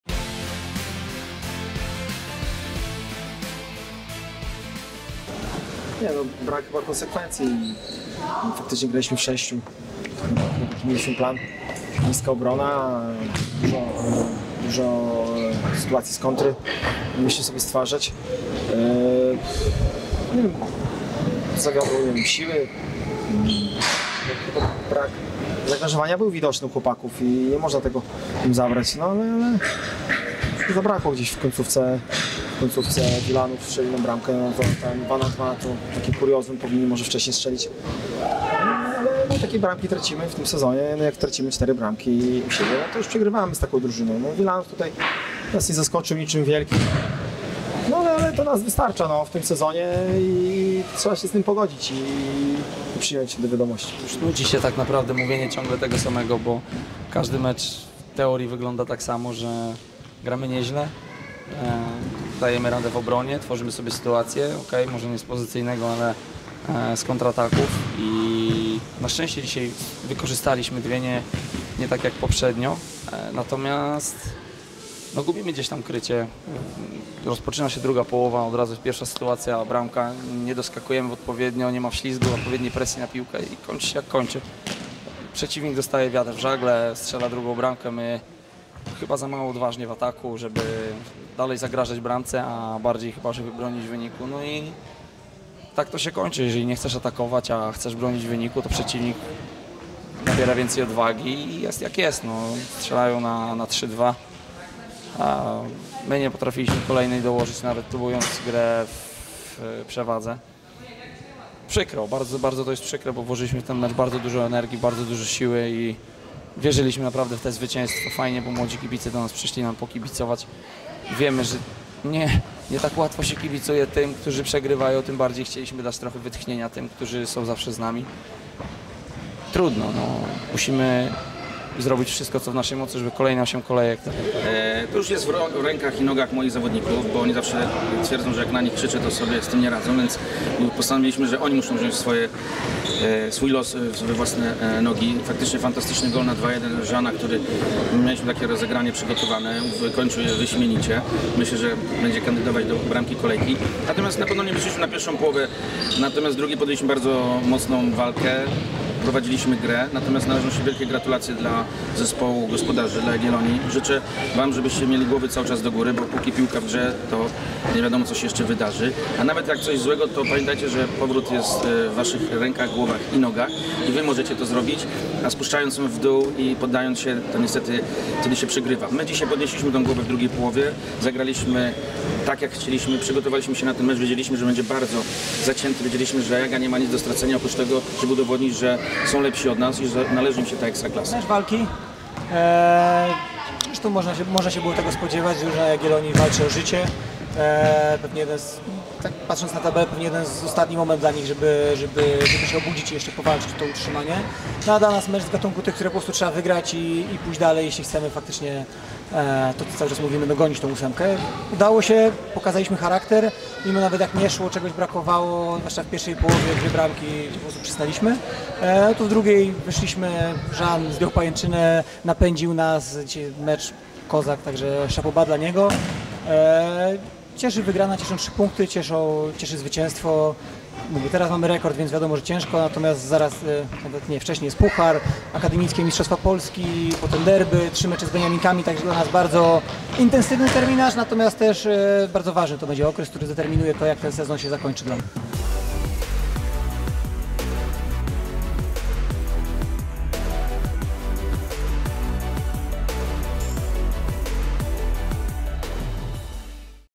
Jagiellonia Białystok - AZS UW DARKOMP 2:4 | WYPOWIEDZI | 22. kolejka (2023/24)
Jagiellonia Białystok vs. AZS UW DARKOMP Wilanów 2:4 (2:0) | 22. kolejka - sezon 2023/24

BRAMKI: Radosław Marcinkowski 4 (s), Piotr Skiepko 19 - Jean Carlos 21, Radosław Marcinkowski 34, Maksym Panasenko 37, 40.

Po meczu wypowiedzieli się trenerzy Karol Domalewski i Maciej Karczyński oraz zawodnicy Grzegorz Makal i Radosław Marcinkowski.

Oglądaj wybrane MECZE KOLEJKI w TVP Sport!

Oglądaj mecze FOGO Futsal Ekstraklasy w każdy weekend na platformie Pilot WP